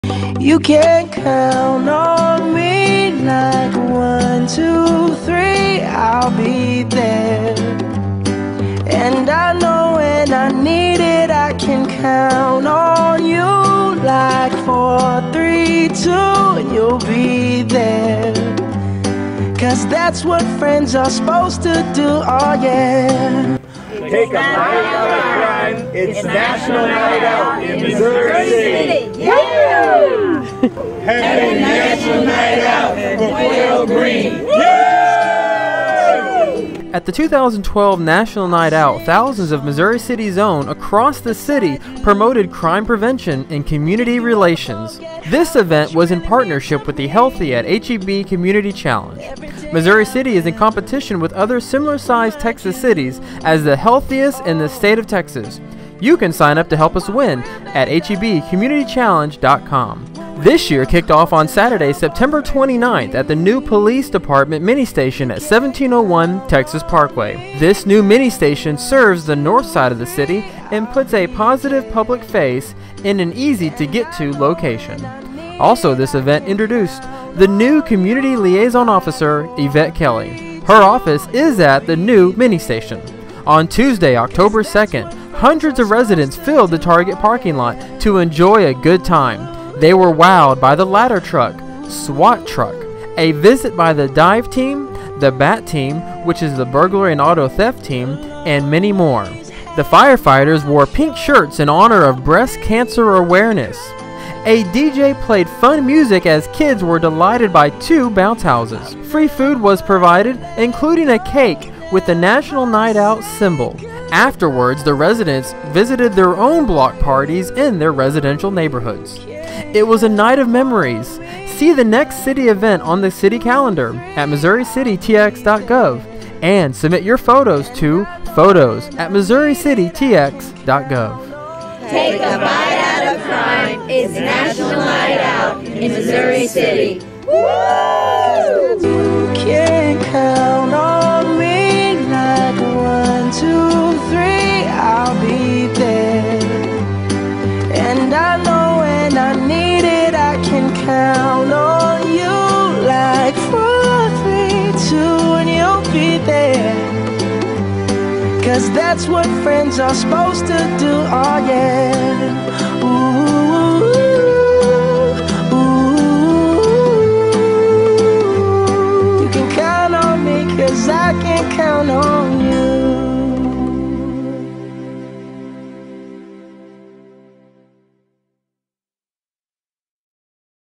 National Night Out 2012 in Missouri City, Texas
Share your National Night Out stories on Twitter. Our handle is @MissouriCityTX.
This event was to help promote a healthier lifestyle with the Healthy at HEB Community Challenge.